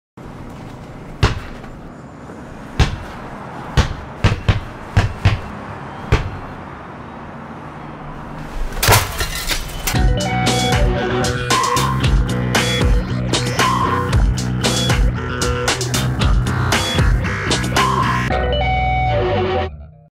SPOT REKLAMOWY SCV2009
Reklama turnieju STREETBALL CHALLENGE VESTAS 2009. Formuła GRAND PRIX, każdy może przyjść i zagrać. Turniej zrealizowano w 2009 roku.